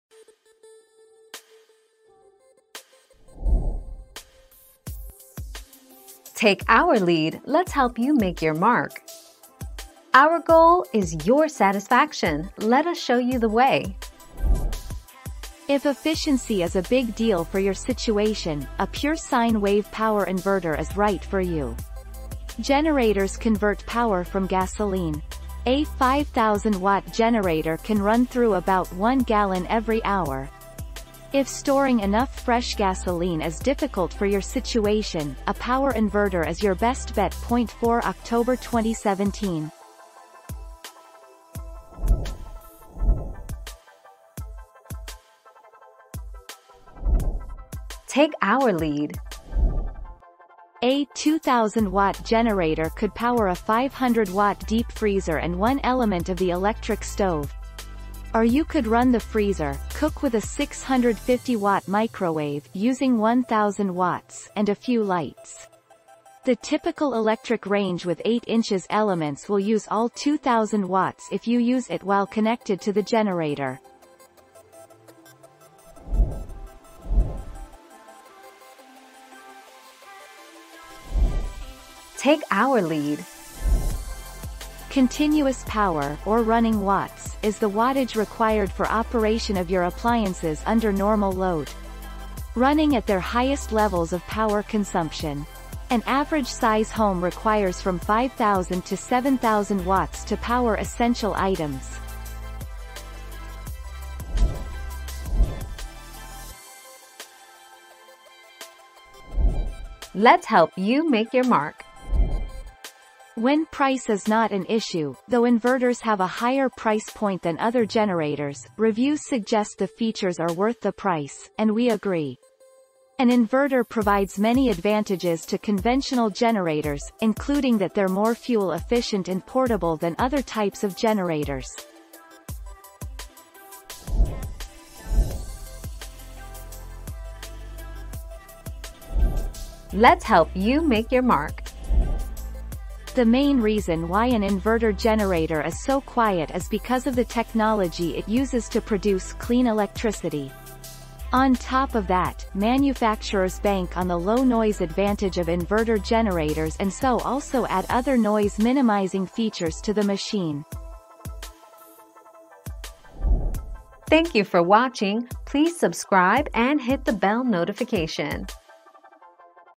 Which is better inverter or generator?
00:00 - Which is better inverter or generator?
00:43 - What will a 2000 watt inverter generator run?
01:17 - How many watts do you need to power a house?
01:45 - Are inverter generators worth the money?
02:15 - Why are Honda generators so quiet?

Laura S. Harris (2021, February 22.) Which is better inverter or generator?
    AskAbout.video/articles/Which-is-better-inverter-or-generator-221603